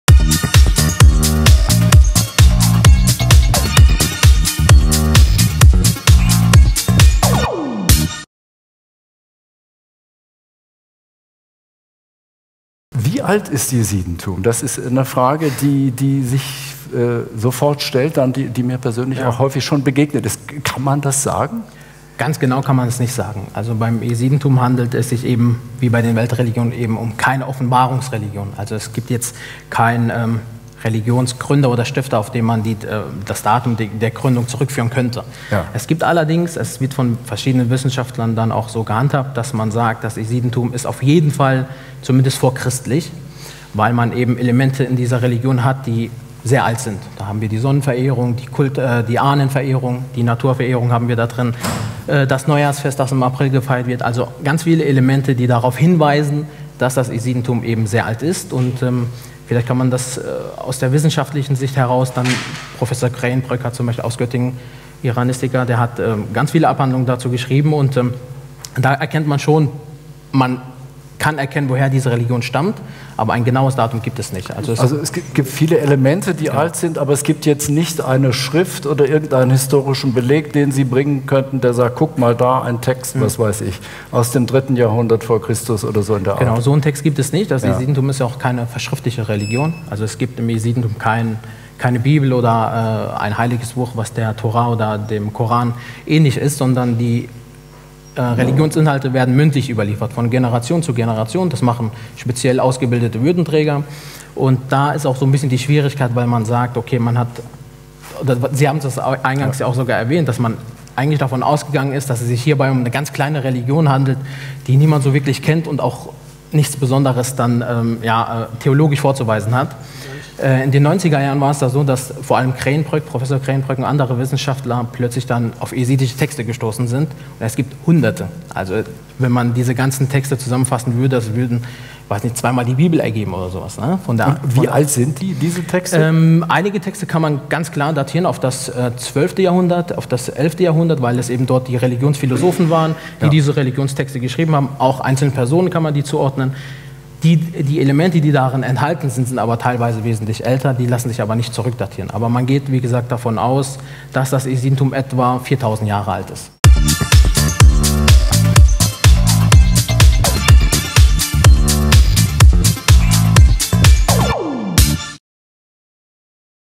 Wie alt ist das Jesidentum?
Von der Öffentlichkeit kaum bemerkt, hat sich Niedersachsen in den letzten Jahren zu einem Zentrum jesidischen Lebens entwickelt. Viele Tausend Jesiden leben insbesondere in den Regionen Hannover, Oldenburg und Celle.

Wer sind die Jesiden? Was sollten Politik und Gesellschaft über Jesidentum wissen? Darüber spricht Wolfgang Reinbold mit dem Bildungsreferenten des yezidischen Forums Oldenburg, Ilyas Yanc, dem Gründer des Nachrichtenportals Ezidipress, Hayri Demir, und mit dem evangelischen Theologen und Jesiden-Experten Dr. Andreas Flick aus Celle.

Gespräch am 17. September 2015 im Haus der Religionen in Hannover.